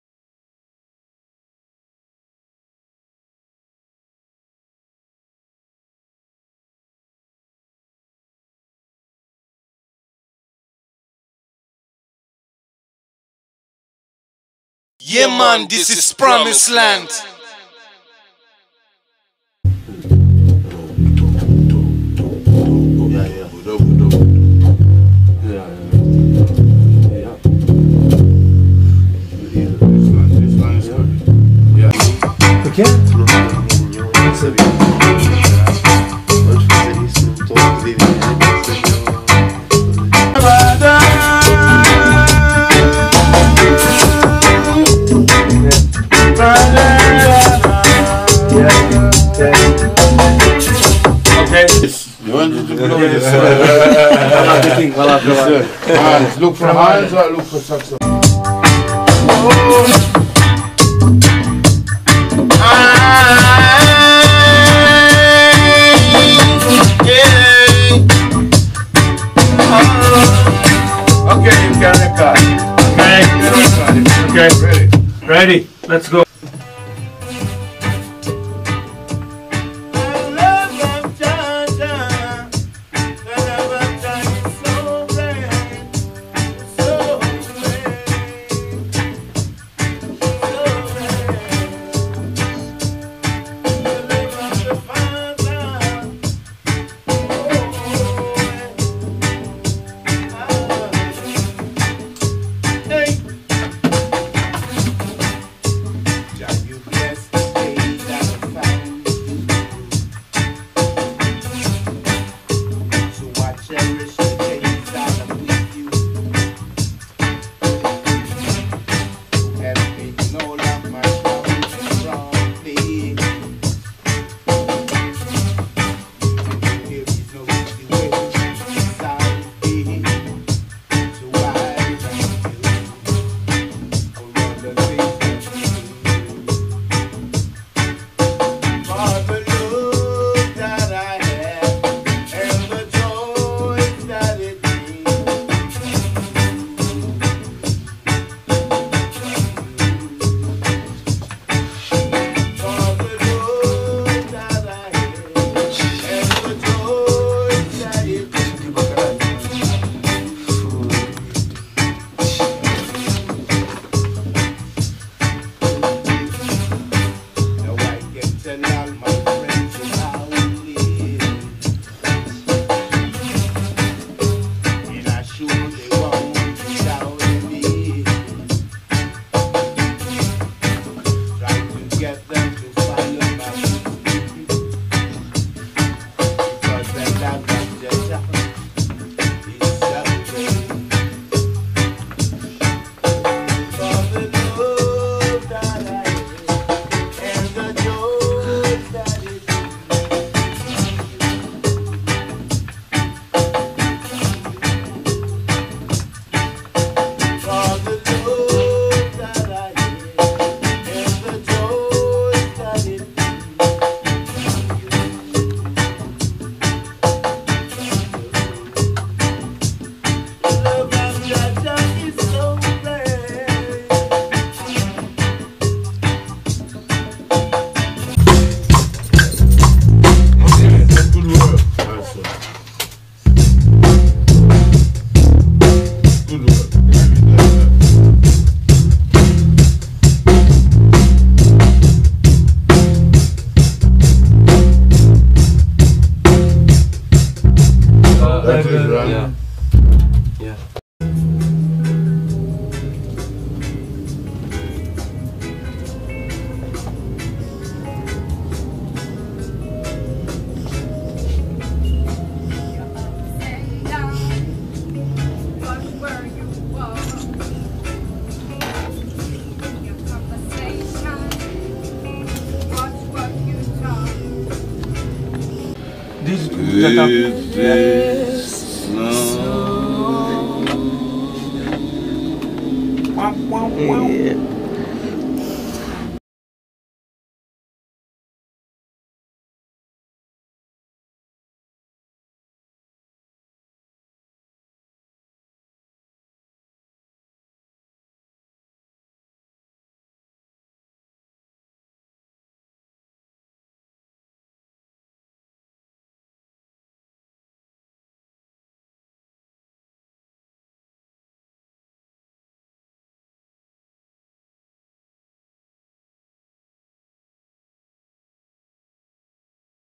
Easton Clarke (JA) nos Estúdios da Promised Land
O artista roots jamaicano, Easton Clarke, que gravou nos estudios de Lee Perry em 1974, esteve, em exclusivo nos Estudios da PROMISED LAND, em gravações.

Brevemente Novidades!!